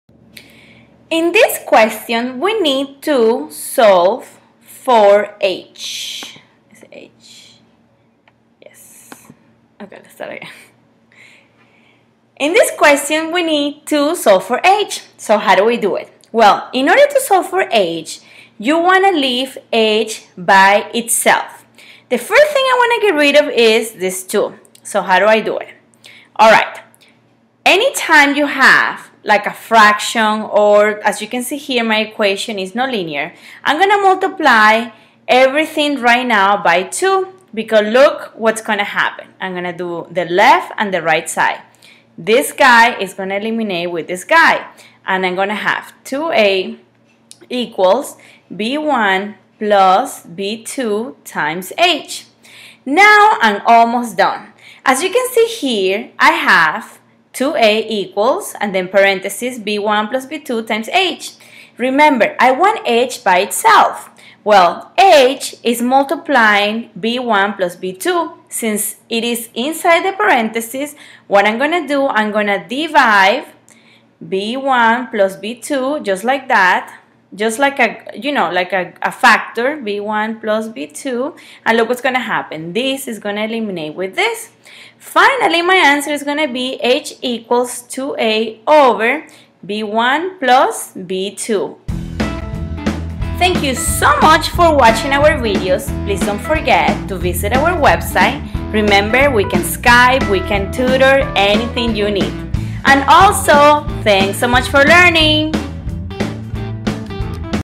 Solving for H in the area trapezoid formula Pet test .mp4
Hi Guys, this video will teach you how to solve equations with letter such as the area of a trapezoid. for tutorials, quizzes, tutoring and more !